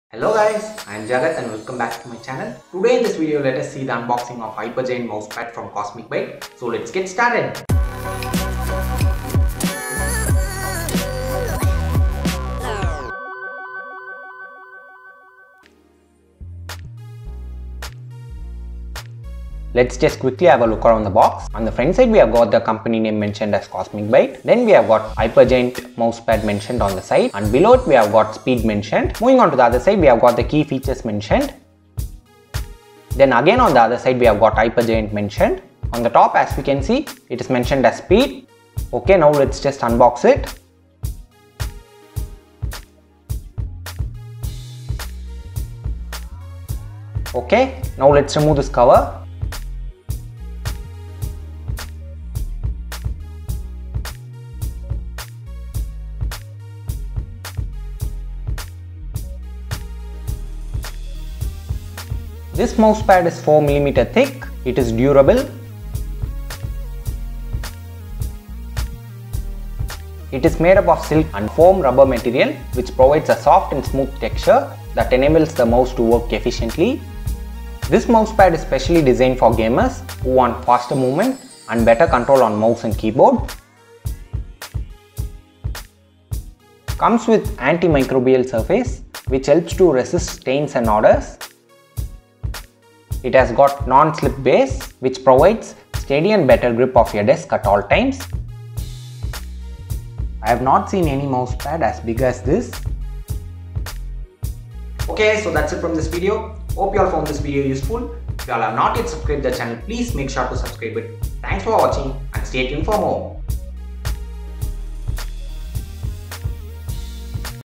CosmicByte|HyperGiant|Mouse-Pad|Unboxing
Hi Guyz, In this video I have unboxed mouse-pad from CosmicByte which is designed specially for Gamers!!Watch the video 2 Know More!
My Tamil Channel:
DON'T FORGET 2
Msg me in Twitter @jagathjak
Mouse-Pad